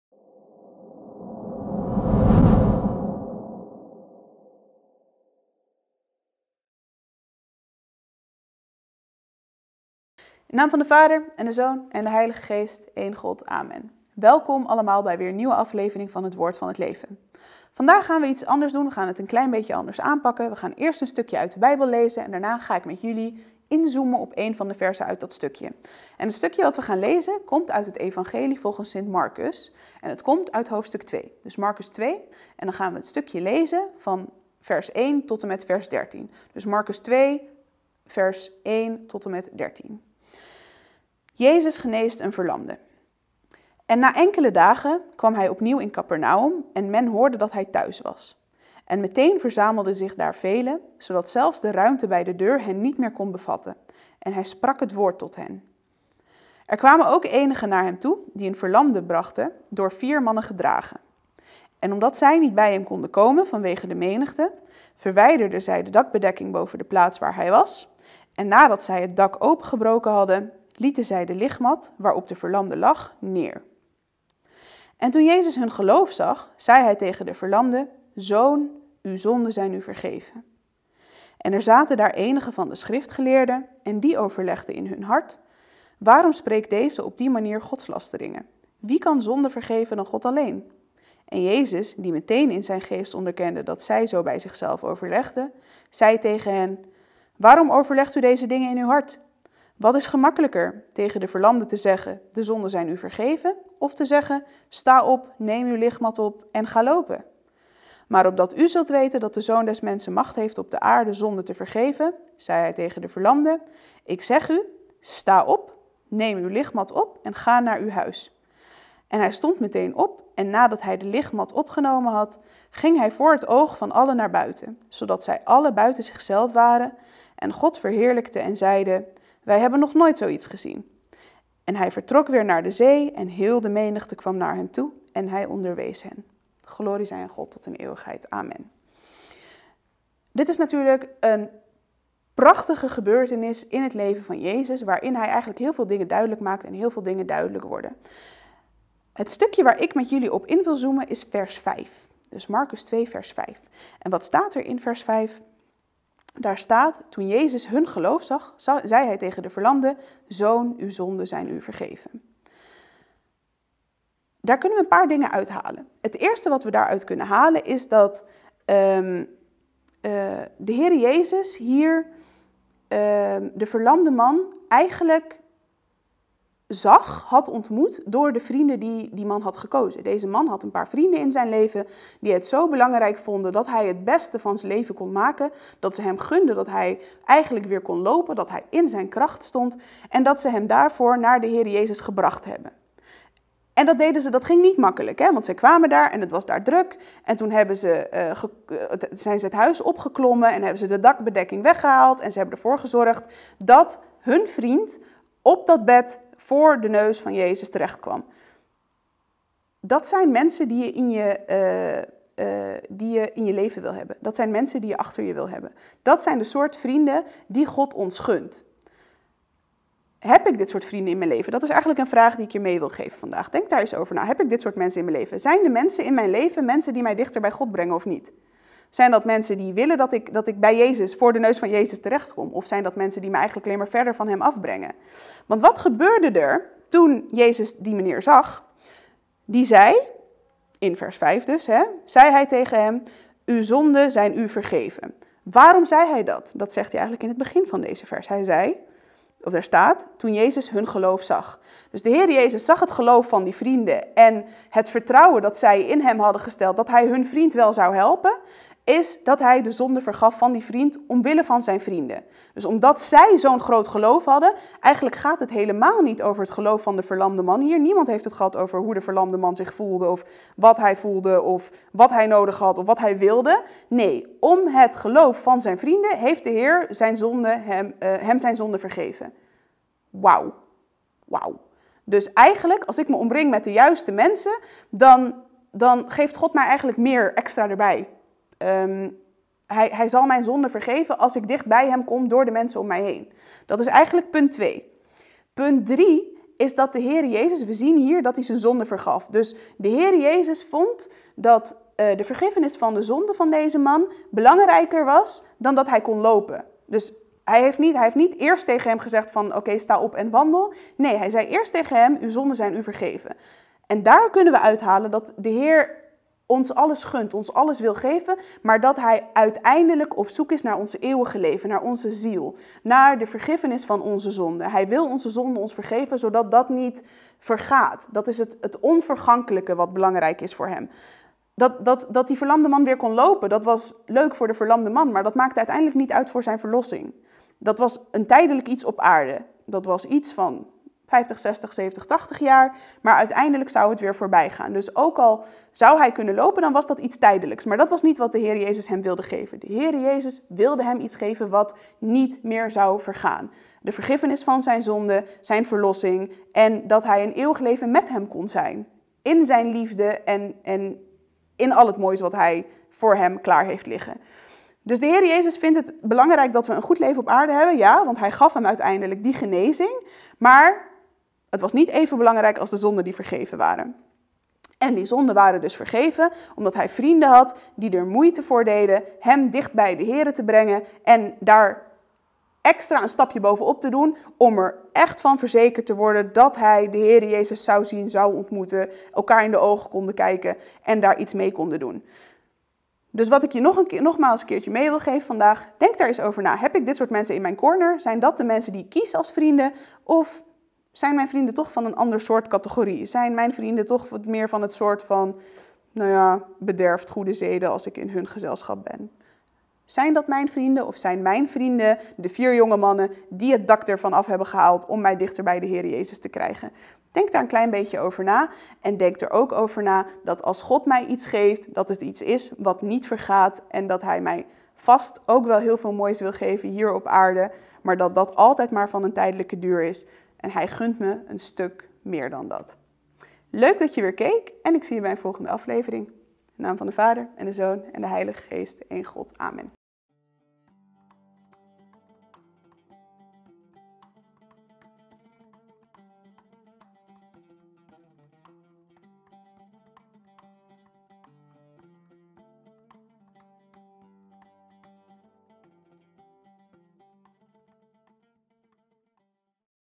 En toen Jezus hun geloof zag, zei Hij tegen de verlamde: Zoon, uw zonden zijn u vergeven
Dienares Irini Sorial
“Aflevering 43 -
 1En na enkele dagen kwam Hij opnieuw in Kapernaüm; en men hoorde dat Hij thuis was.
2En meteen verzamelden zich daar velen, zodat zelfs de ruimte bij de deur hen niet meer kon bevatten; en Hij sprak het Woord tot hen.
3Er kwamen ook enigen naar Hem toe die een verlamde brachten, door vier mannen gedragen.
4En omdat zij niet bij Hem konden komen vanwege de menigte, verwijderden zij de dakbedekking boven de plaats waar Hij was; en nadat zij het dak opengebroken hadden, lieten zij de ligmat waarop de verlamde lag, neer.
5En toen Jezus hun geloof zag, zei Hij tegen de verlamde: Zoon, uw zonden zijn u vergeven.
6En er zaten daar enigen van de schriftgeleerden, en die overlegden in hun hart:
7Waarom spreekt Deze op die manier godslasteringen? Wie kan zonden vergeven dan God alleen?
8En Jezus, Die meteen in Zijn geest onderkende dat zij zo bij zichzelf overlegden, zei tegen hen: Waarom overlegt u deze dingen in uw hart?
9Wat is gemakkelijker, tegen de verlamde te zeggen: De zonden zijn u vergeven, of te zeggen: Sta op, neem uw ligmat op en ga lopen?
10Maar opdat u zult weten dat de Zoon des mensen macht heeft op de aarde zonden te vergeven (zei Hij tegen de verlamde):
11Ik zeg u: Sta op, neem uw ligmat op en ga naar uw huis.
12En hij stond meteen op, en nadat hij de ligmat opgenomen had, ging hij voor het oog van allen naar buiten, zodat zij allen buiten zichzelf waren en God verheerlijkten en zeiden: Wij hebben nog nooit zoiets gezien! En Hij vertrok weer naar de zee; en heel de menigte kwam naar Hem toe, en Hij onderwees hen